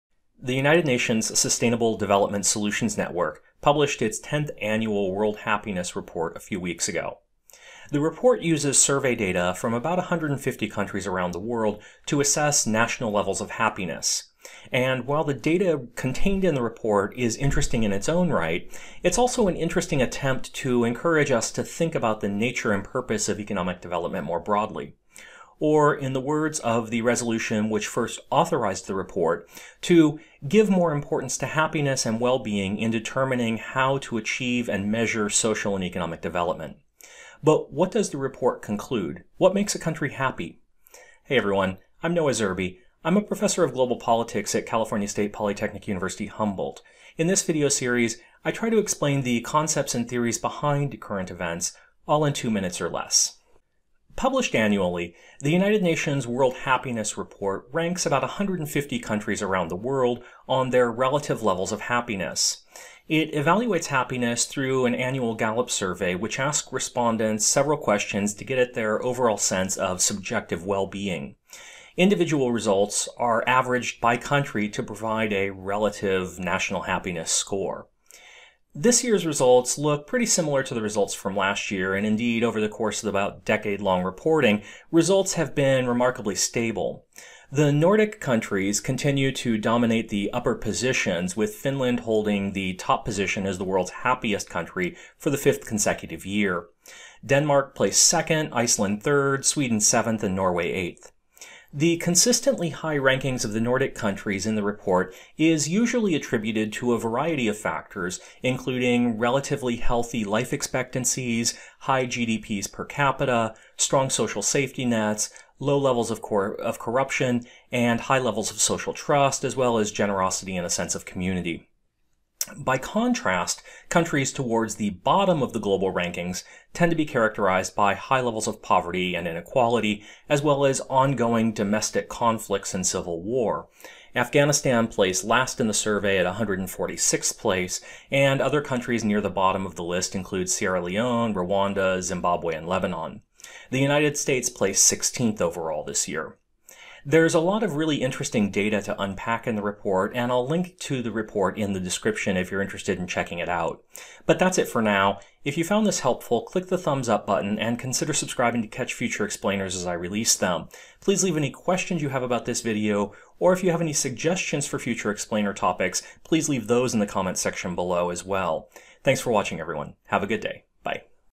What Makes a Country Happy? How Are Happiness and Development Related?
The annual World Happiness Report was recently published, and the results were pretty much in line with what they had been for the last several reports. But what exactly makes a country happy? And how does the idea of happiness relate to the concept of development? Find out in this Two Minute Explainer!